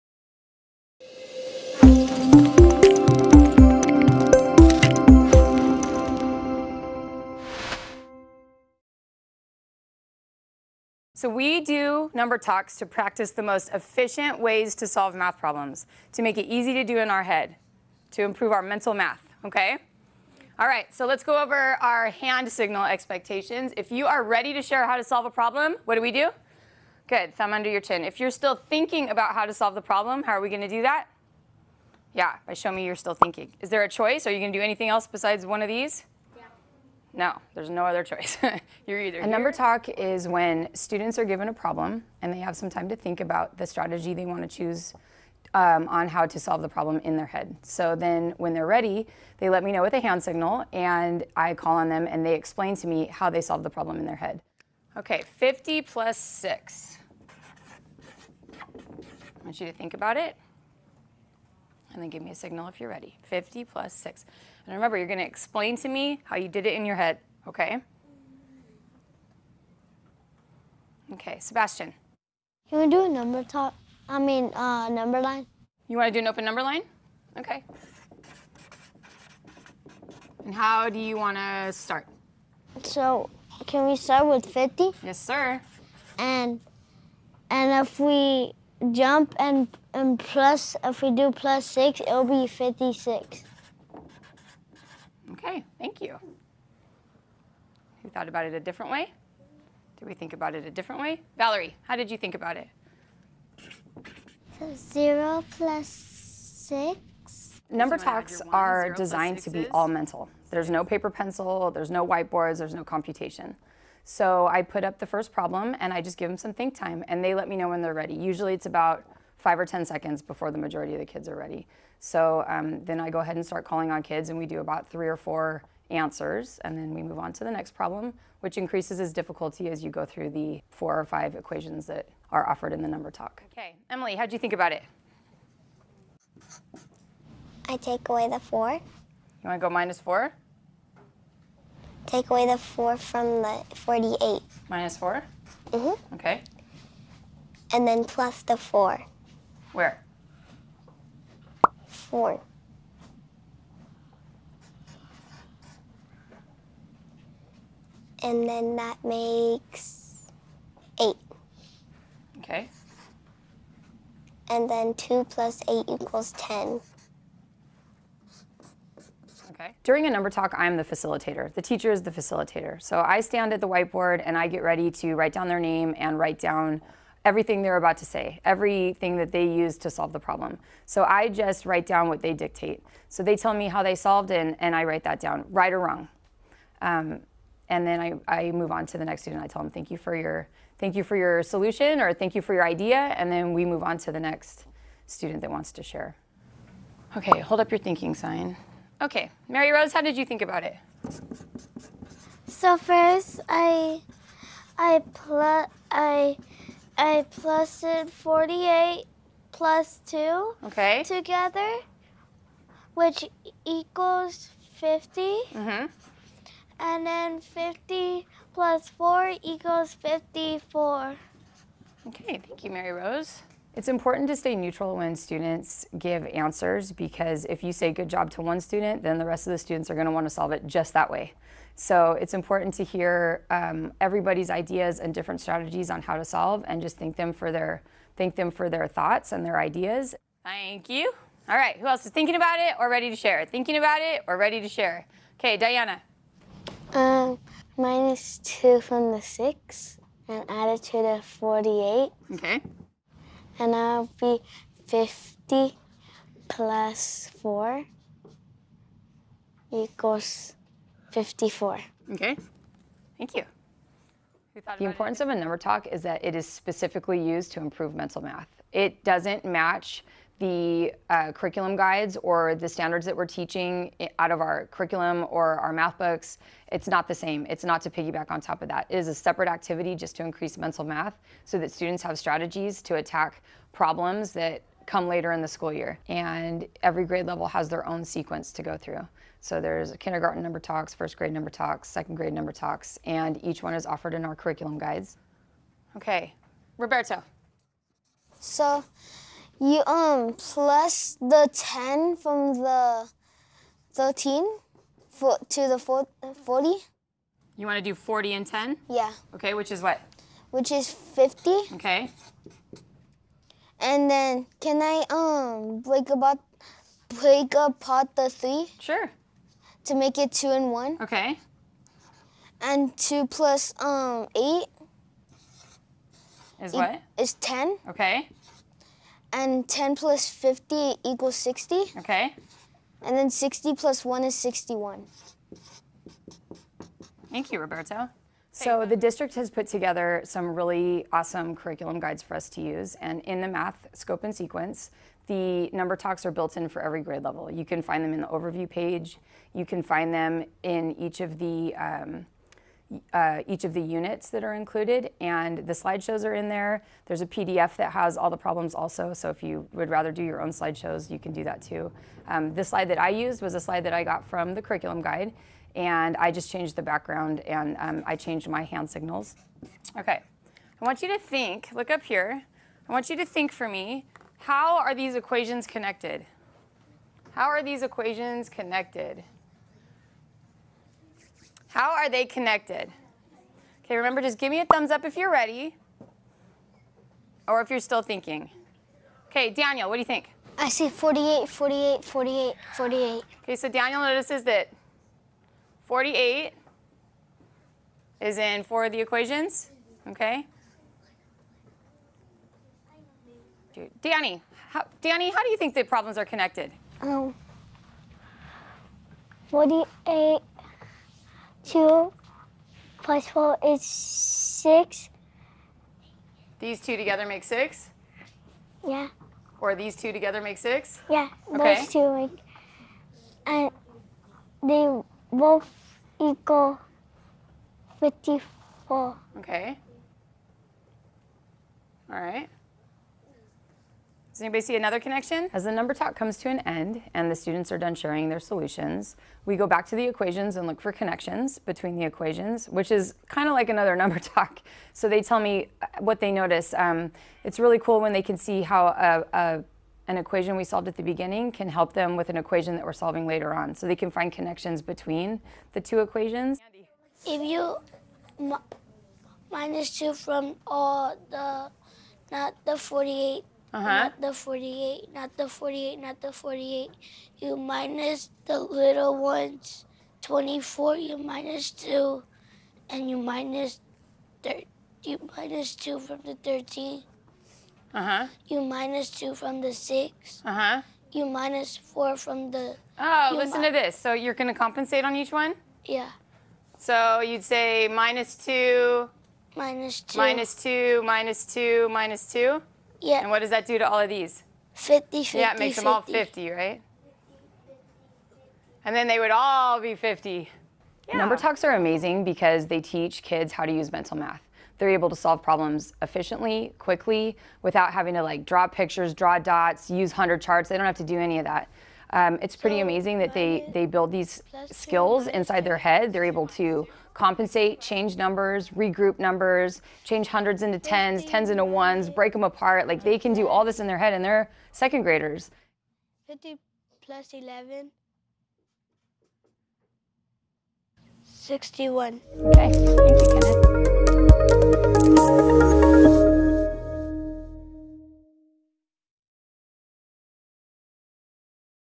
Using Math Number Talks to Develop Mental Math
Demonstration Teacher Kristi Leader showcases how she teaches her second grade students mental math strategies as part of her math time block. Watch the classroom dynamic as her students engage with a series of problems, exchanging insights under Ms. Leader's facilitation as they draw connections and develop their mental math strategies.